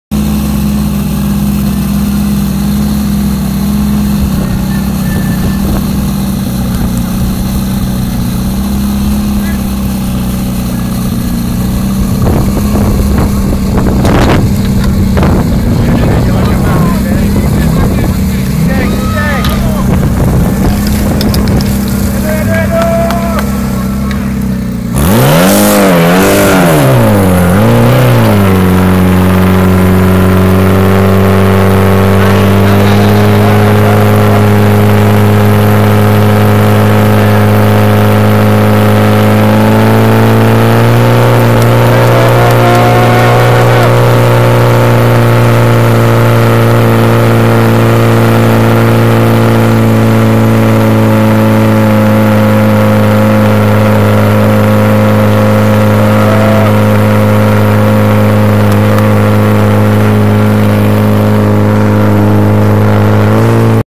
O pohár starosty Města Libčice nad Vltavou 2014: SDH Libčice ženy B (2. kolo)
Požární útok družstva na závodech O pohár starosty Města Libčice nad Vltavou 2014.

SDH Libčice ženy B
2. Kolo. Čas: 47:59